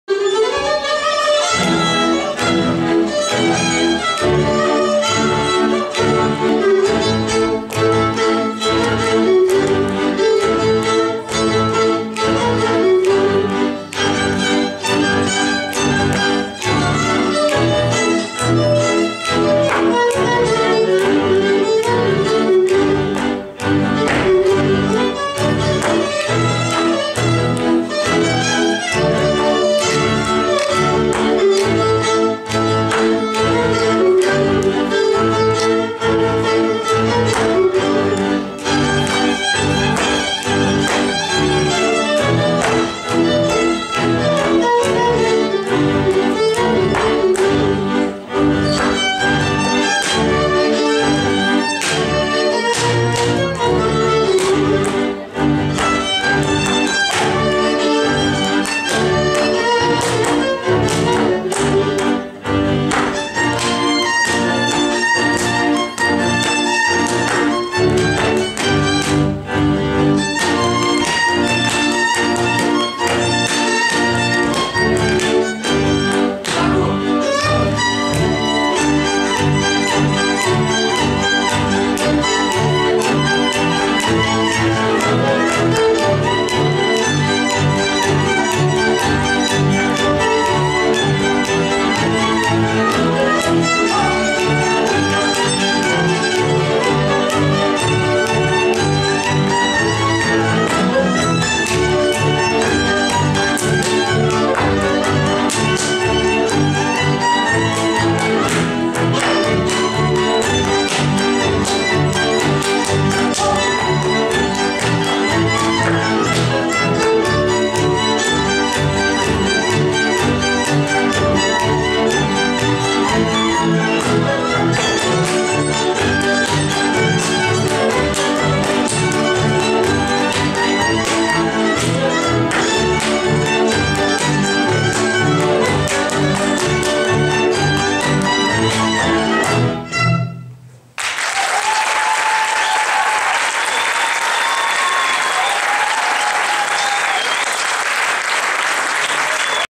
21. Donovál Márton: Fecioreste romîneste sî bărbunc (Ördöngősfüzes)
3. korcsoport
Szabadonválasztott táncok
V. Országos Néptáncverseny (páros és szóló)
Regionális válogató
Siófok, 2015. április 15.
Kísér: Somogy zenekar